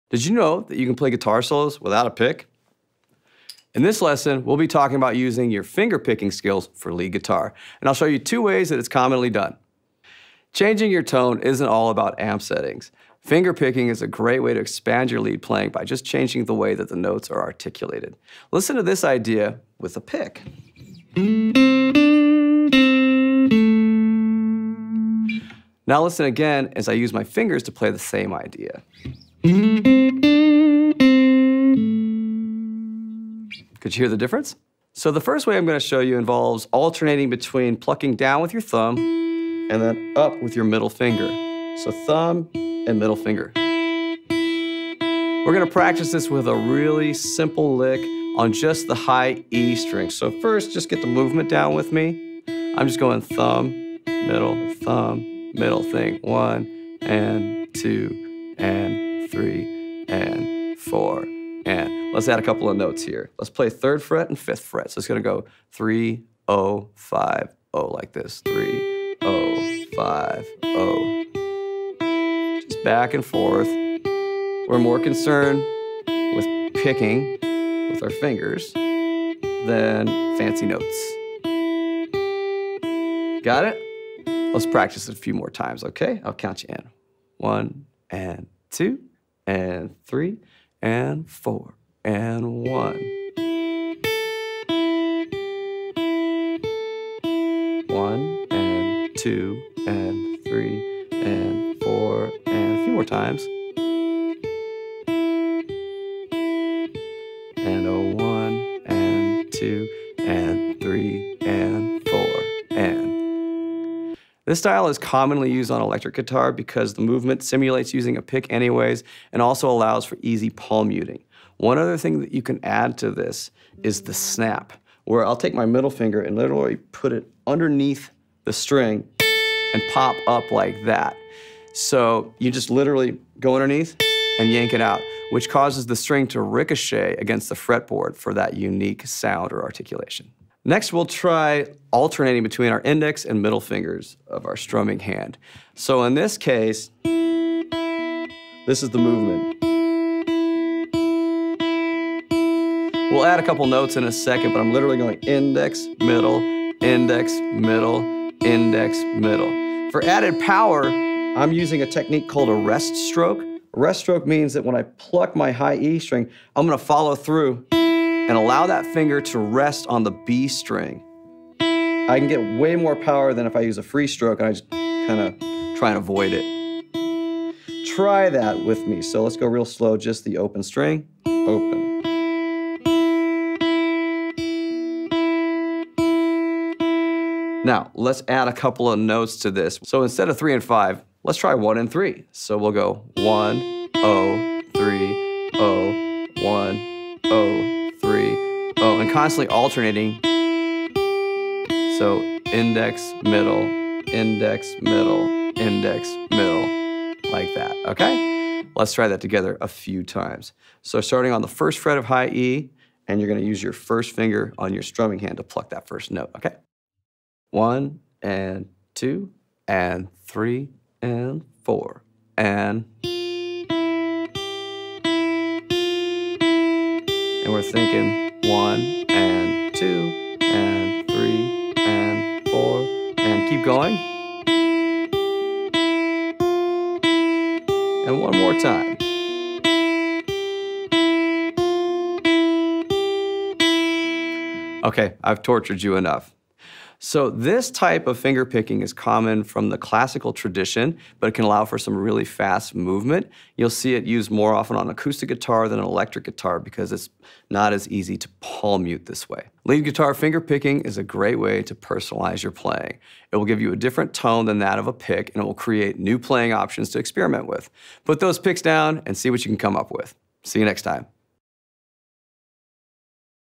How to use Fingerpicking for Lead Guitar | Fender Play
Did you know you can play guitar solos without a pick? Watch as Fender Play instructor Matt Lake shows you how to use fingerpicking while playing lead guitar. To learn hundreds of other songs, skills, and exercises